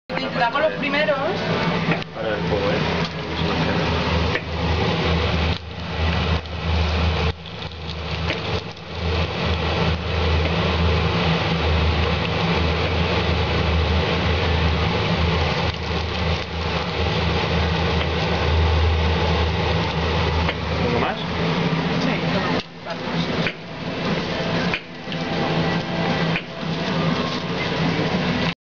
¡Otro pan de gambas!
Cómo hacer pan de gambas. Artistas invitados: roz y chulín.